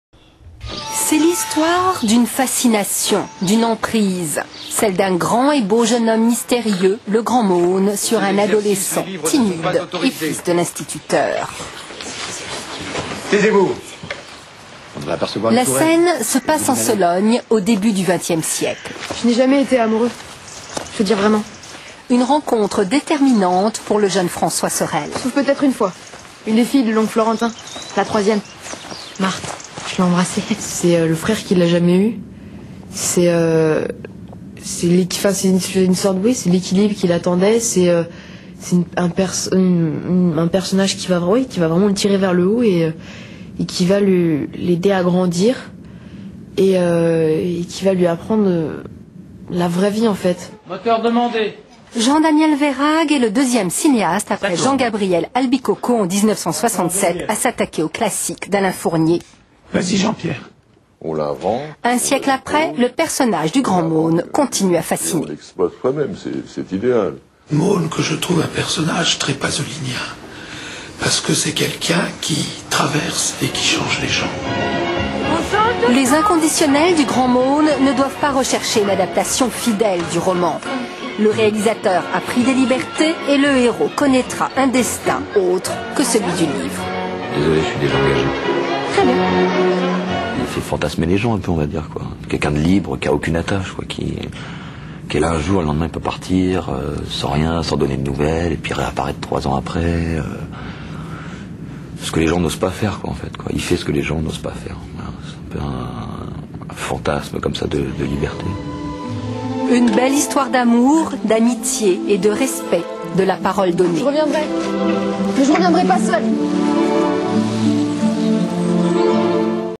Jean-Baptiste Maunier Interview Le Grand Meaulnes
Jean-Baptiste Maunier au journal télévisé de France 3 le 4 octobre 2006 pour la sortie au cinéma du film Le Grand Meaulnes

Retrouvez plus d'infos, d'actu, de photos et de vidéos sur le site officiel